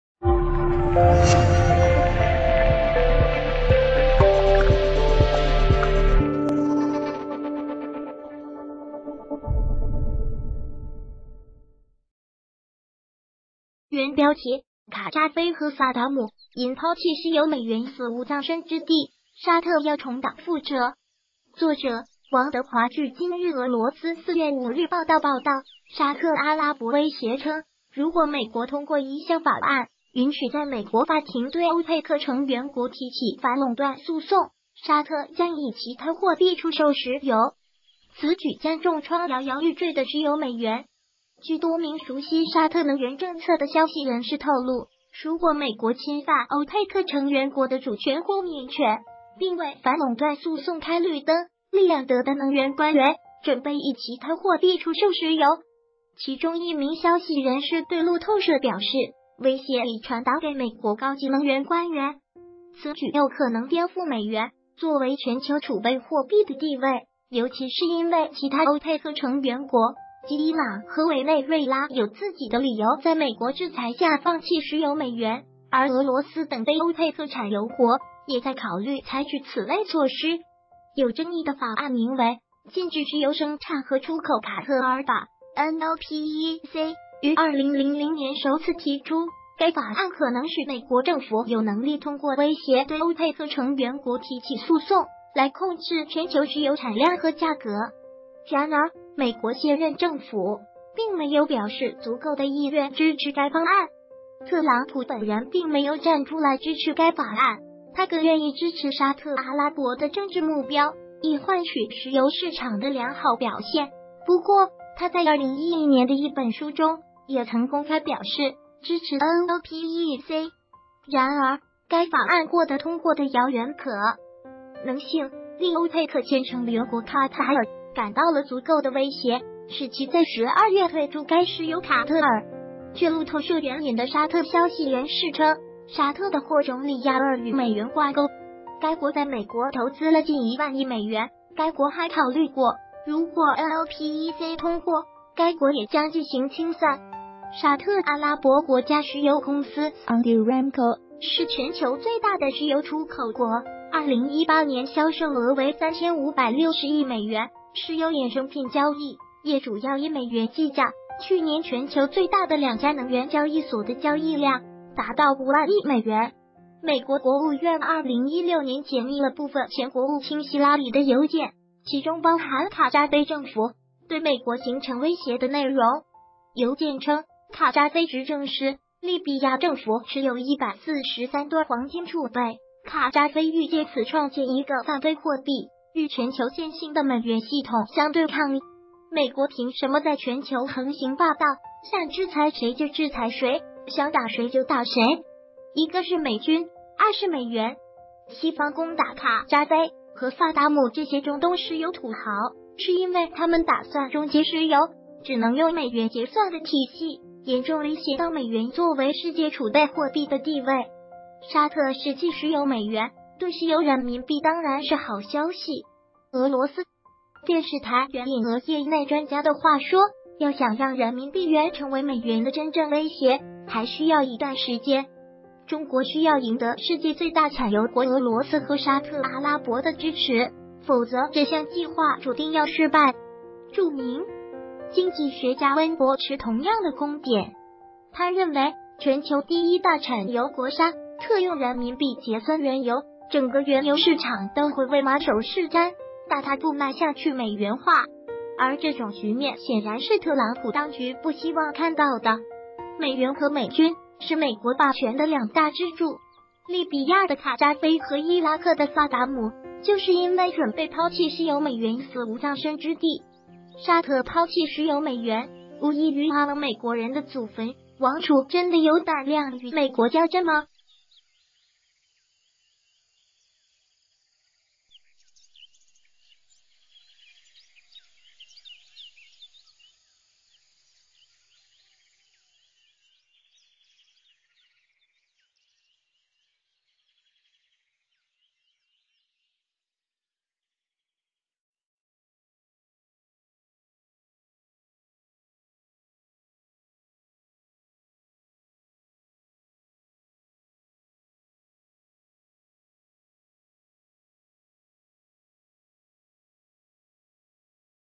卡扎菲和萨达姆因抛弃石油美元死无葬身之地，沙特要重蹈覆辙？_欧佩克
谢谢！原标题：卡扎菲和萨达姆因抛弃石油美元死无葬身之地，沙特要重蹈覆辙？作者：王德华据《今日俄罗斯》4月5日报道报道，沙特阿拉伯威胁称，如果美国通过一项法案，允许在美国法庭对欧佩克成员国提起反垄断诉讼，沙特将以其它货币出售石油。此举将重创摇摇欲坠的石油美元。据多名熟悉沙特能源政策的消息人士透露，如果美国侵犯欧佩克成员国的主权豁免权，并为反垄断诉讼开绿灯，利雅得的能源官员准备以其他货币出售石油。其中一名消息人士对路透社表示，威胁已传达给美国高级能源官员。此举有可能颠覆美元作为全球储备货币的地位，尤其是因为其他欧佩克成员国——即伊朗和委内瑞拉——有自己的理由在美国制裁下放弃石油美元，而俄罗斯等非欧佩克产油国也在考虑采取此类措施。有争议的法案名为《禁止石油生产和出口卡特尔法》(NOPEC)，于2000年首次提出，该法案可能使美国政府有能力通过威胁对欧佩克成员国提起诉讼，来控制全球石油产量和价格。然而，美国现任政府，并没有表示足够的意愿支持该方案。特朗普本人并没有站出来支持该法案，他更愿意支持沙特阿拉伯的政治目标，以换取石油市场的良好表现，不过他在2011年的一本书中也曾公开表示支持NOPEC。然而，该法案获得通过的遥远可能性，令欧佩克前成员国卡塔尔感到了足够的威胁，使其在12月退出该石油卡特尔。据路透社援引的沙特消息人士称，沙特的货种里亚尔与美元挂钩。该国在美国投资了近1万亿美元，该国还考虑过，如果NOPEC通过，该国也将进行清算。沙特阿拉伯国家石油公司(SaudiAramco)是全球最大的石油出口国，2018年销售额为3560亿美元，石油衍生品交易也主要以美元计价，去年全球最大的两家能源交易所的交易量达到5万亿美元。美国国务院2016年解密了部分前国务卿希拉里的邮件，其中包含卡扎菲政府对美国形成威胁的内容。邮件称，卡扎菲执政时，利比亚政府持有143吨黄金储备，卡扎菲欲借此创建一个泛非货币，与全球现行的美元系统相对抗。美国凭什么在全球横行霸道，想制裁谁就制裁谁，想打谁就打谁？一个是美军，二是美元。西方攻打卡扎菲和萨达姆这些中东石油土豪，是因为他们打算终结石油只能用美元结算的体系，严重威胁到美元作为世界储备货币的地位。沙特势弃石油美元，对石油人民币当然是好消息。俄罗斯RT电视台援引俄业内专家的话说，要想让人民币元成为美元的真正威胁，还需要一段时间；中国需要赢得世界最大产油国俄罗斯和沙特阿拉伯的支持，否则这项计划注定要失败。著名经济学家温伯持同样的观点。他认为，全球第一大产油国沙特用人民币结算原油，整个原油市场都会唯马首是瞻，大踏步迈向去美元化。而这种局面显然是特朗普当局不希望看到的。美元和美军是美国霸权的两大支柱。利比亚的卡扎菲和伊拉克的萨达姆就是因为准备抛弃石油美元死无葬身之地。沙特抛弃石油美元无异于挖了美国人的祖坟，王储真的有胆量与美国较真吗？